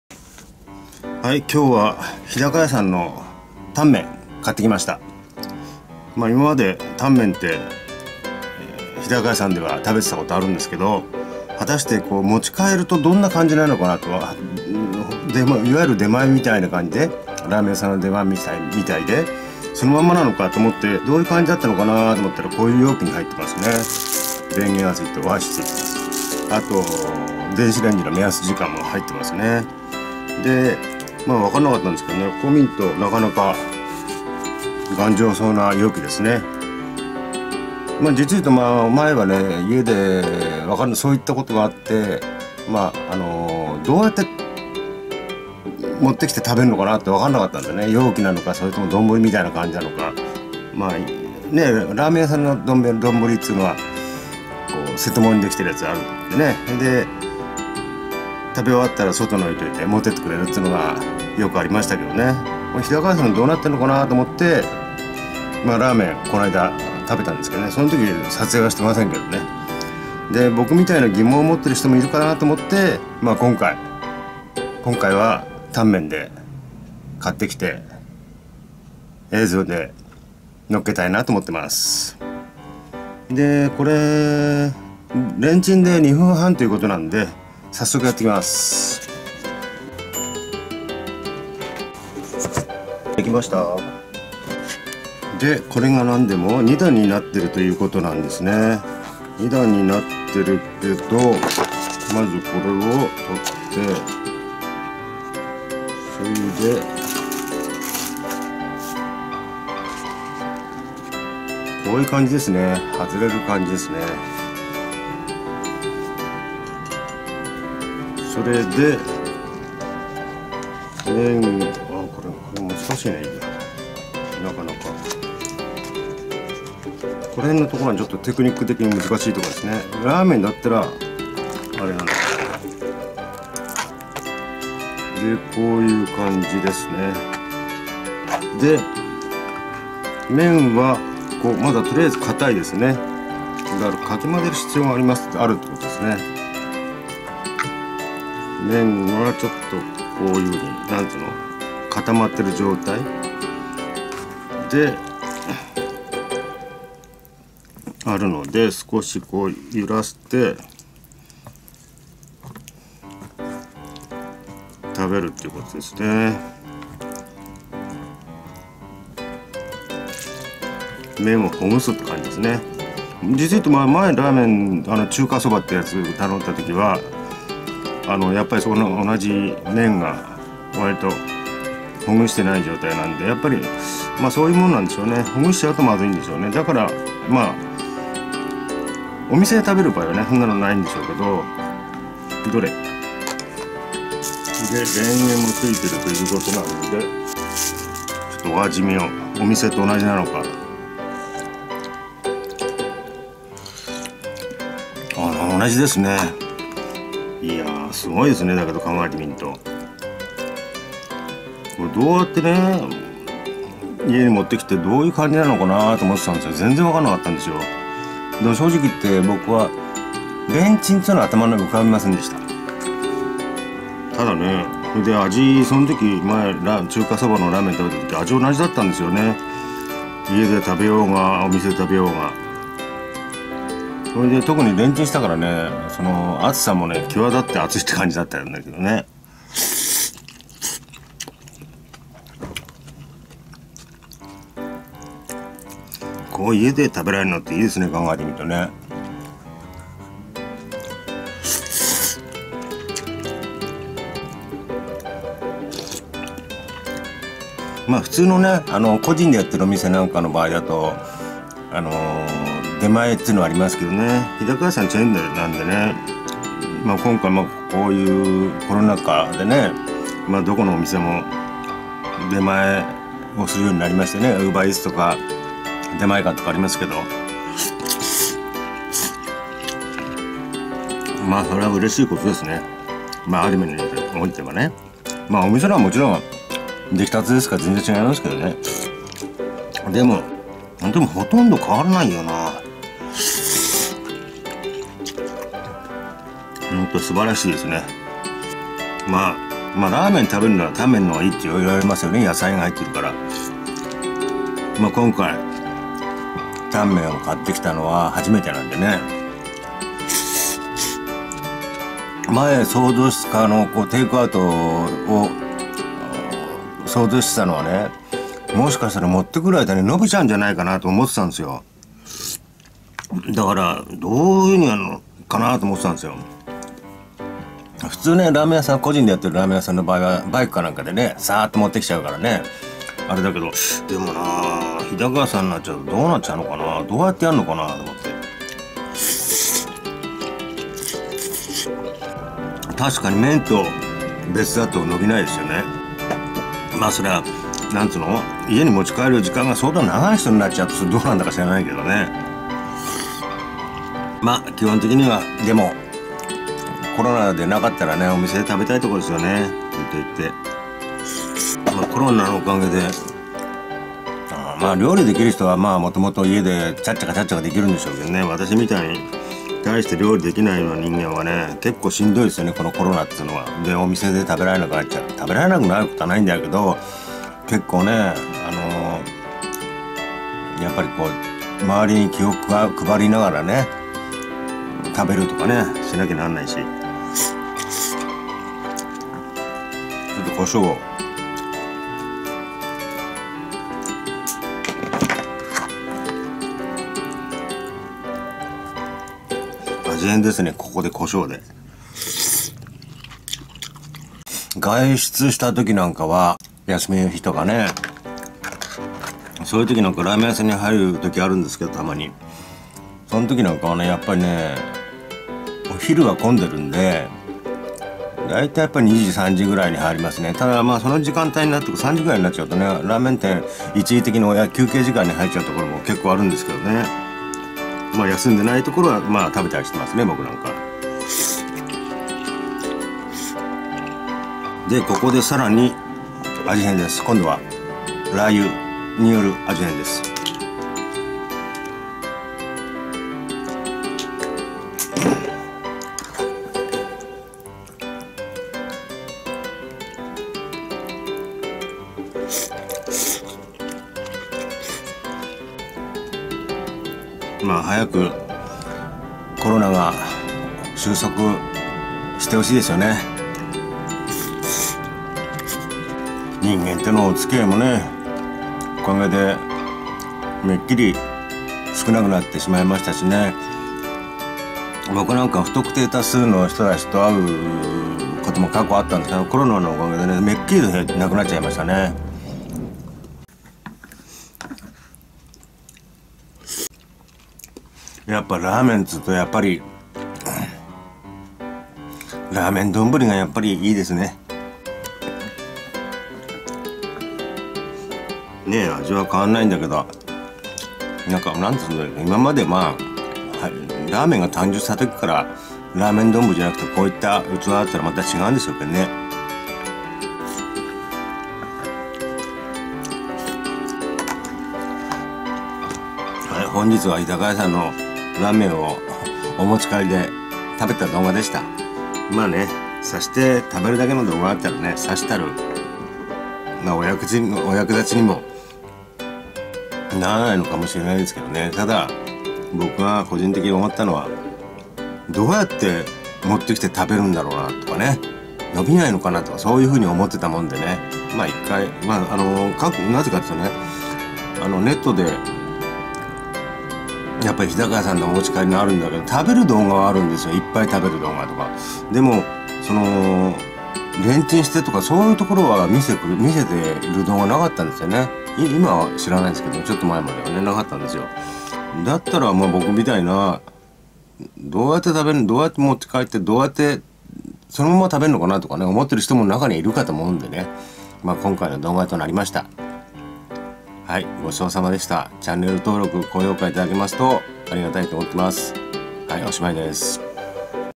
【独身者おひとりさま男の一人飯】熱烈中華食堂日高屋の野菜たっぷりタンメンをお持ち帰り(テイクアウト)してみた！
再生リストをクリックして頂けますと、当該ジャンルの動画をご覧いただけます。

サブチャンネル「レトルトカレーまっちゃんねる」
・・・も、よろしくお願い致します。

BGM・・・甘茶の音楽工房様